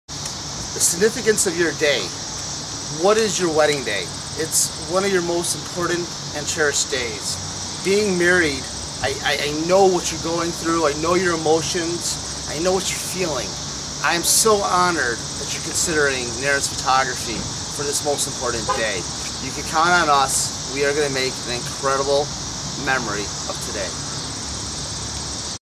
video filmed with bad audio
In this video we can see that the subject was filmed with intense background noise. Try to select sites without wind, traffic, or people noise in the background when doing an interview.